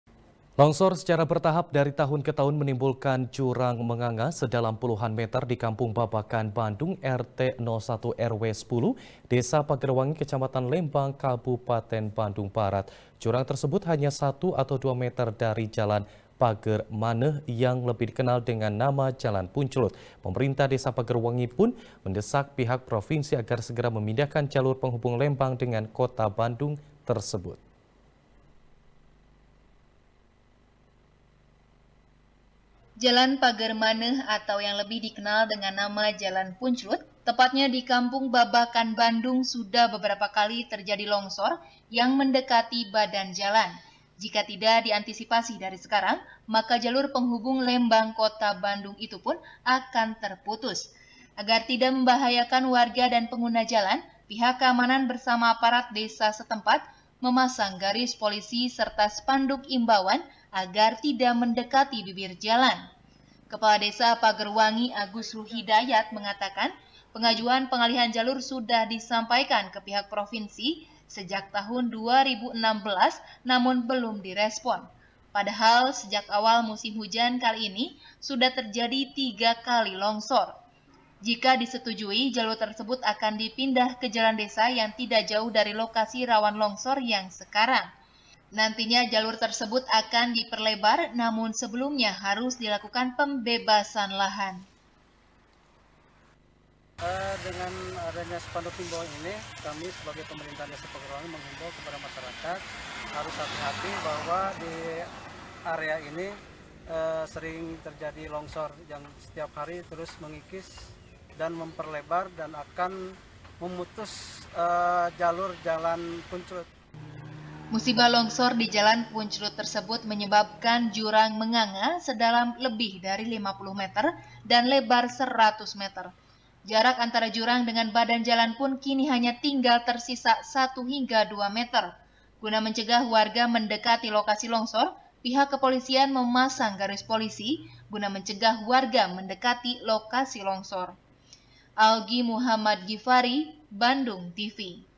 Longsor, Akses Lembang Via Punclut Terancam Terputus | SBR | BANDUNGTV
SBR | BANDUNGTV | IRFAN BUDIAWAN | Longsor secara bertahap dari tahun ke tahun menimbulkan jurang menganga sedalam puluhan meter di kampung babakan bandung,rt 1 rw 10,desa pagerwangi, kecamatan lembang, kabupaten bandung barat. jurang tersebut, hanya satu atau dua meter dari jalan pagermaneuh, yang lebih dikenal dengan nama jalan punclut. pemerintah desa pagerwangi pun mendesak pihak provinsi agar segera memindahkan jalur penghubung lembang dengan kota bandung tersebut.